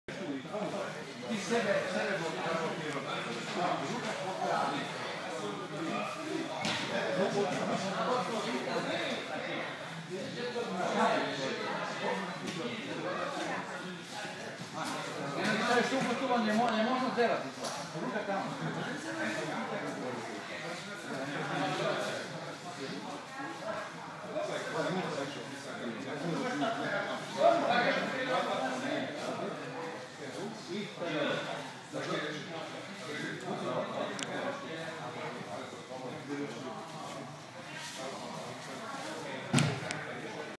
Real Aikido with Ljubomir Vračarević sensei in Israel seminar 2012
Real Aikido (Serbian: Реални аикидо) is a martial art developed by Ljubomir Vračarević, a self-defence instructor from Serbia. It is a mixture of aikido, judo and jujutsu techniques, with some modifications made by Vračarević.